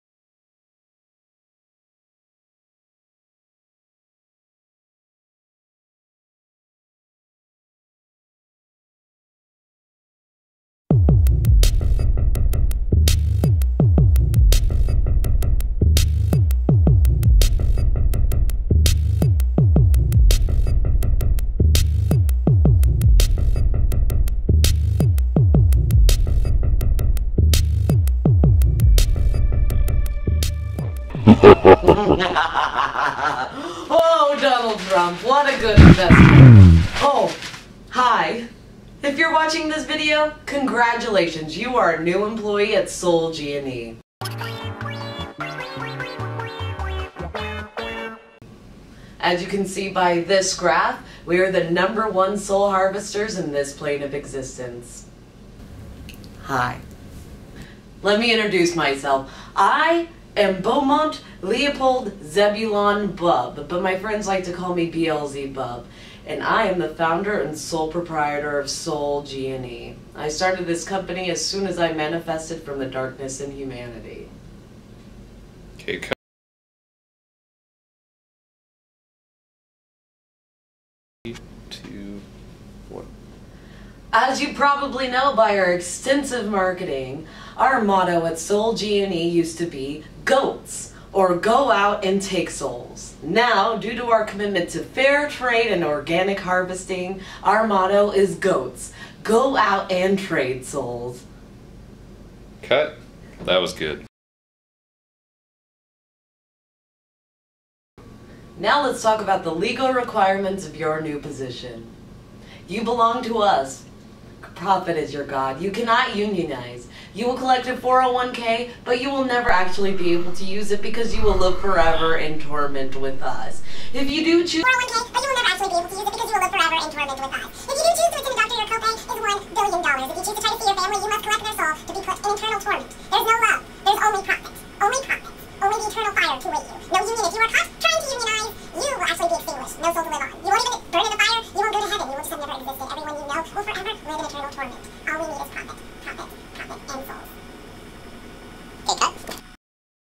Horror Mini-Series Test Pilot: SoulG&E Training Video
My friends and I are in process of making a horror mini-series. Here is a Training video the evil company Soul G&E will be presenting to an intern. Rough edit